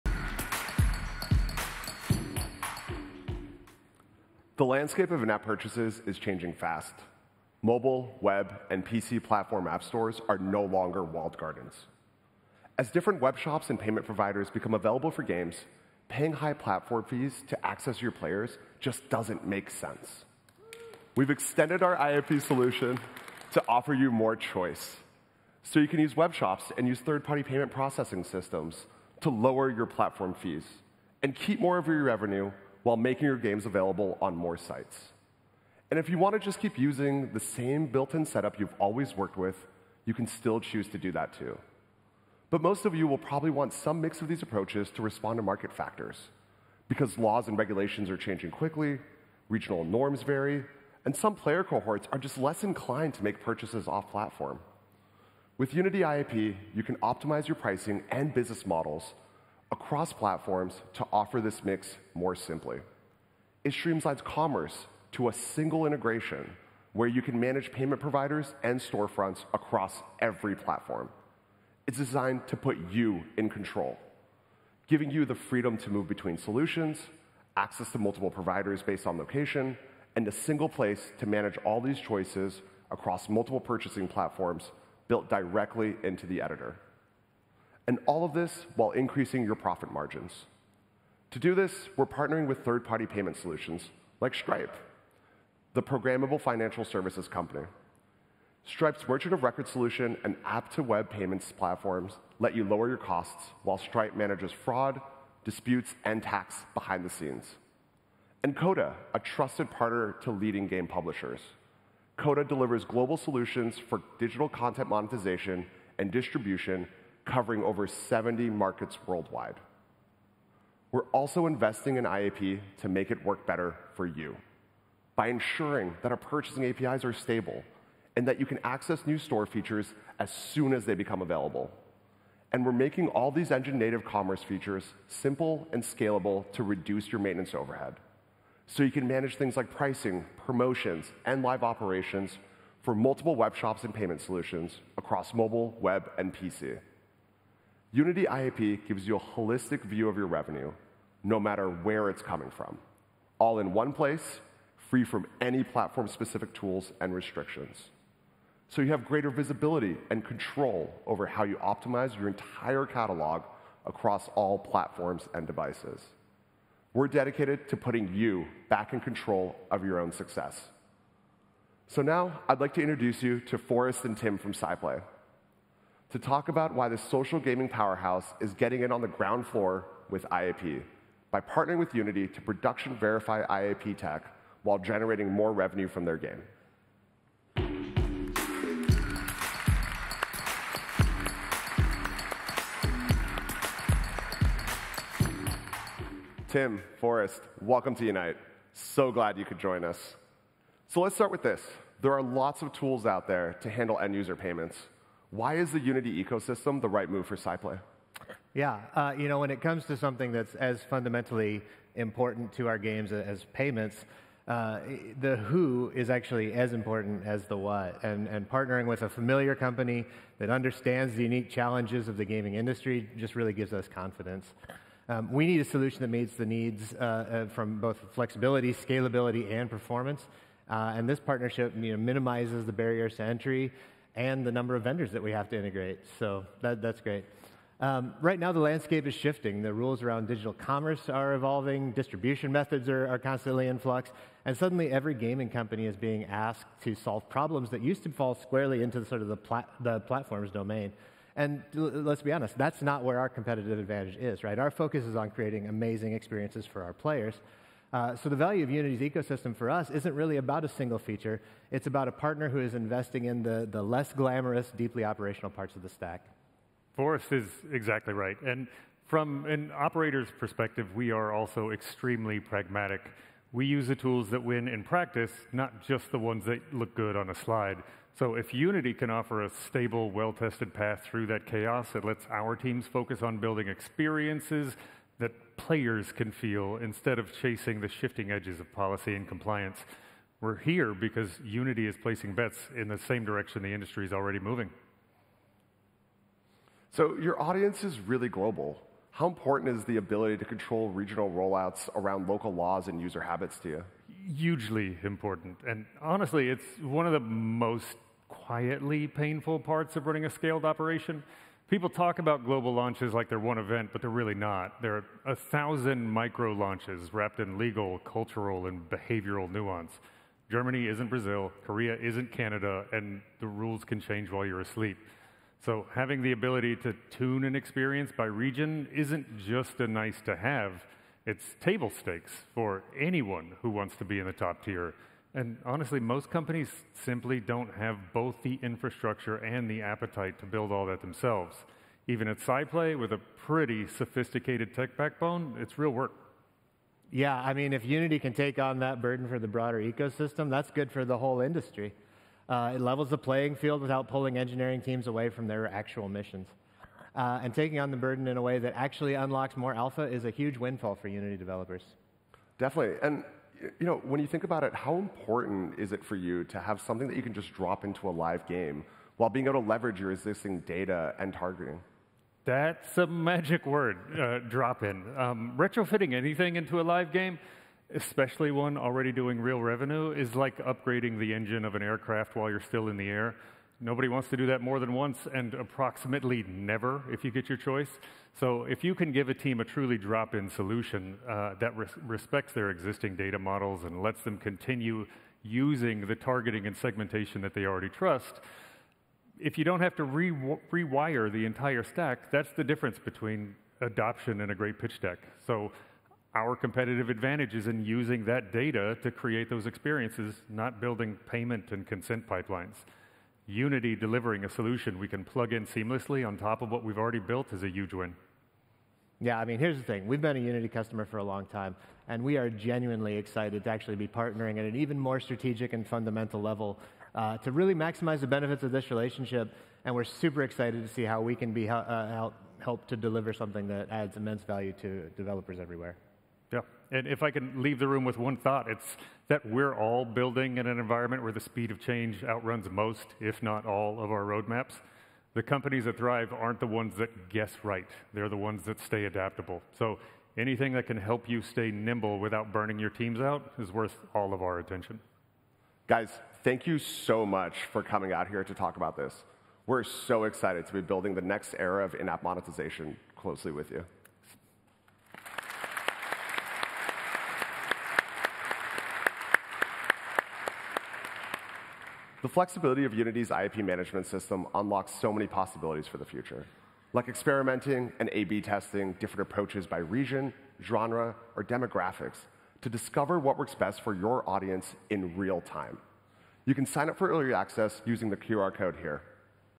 IAP Cross-Platform Commerce Solution | Unite 2025 Keynote
The Unite 2025 Keynote took place November 19, 2025 in Barcelona, Spain, where we shared the latest tech, dev success stories, and exciting new partnerships with the Unity community.

Unity’s Rambod Kermanizadeh shared about how Unity recently extended our In-App Purchasing (IAP) solution with a new cross-platform commerce platform that lets you choose web shops and third-party payment processing systems to lower platform fees and keep more of your revenue. Our system is designed for flexibility, so you can choose how to optimize your pricing and business model using a single integration to manage payment providers and storefronts. We’ve already partnered with payment providers Stripe and Coda, and it will work with games built in both Unity and Unreal Engine. Forrest Stowe and Tim Moore from SciPlay joined Rambod onstage to talk about why they opted in for IAP.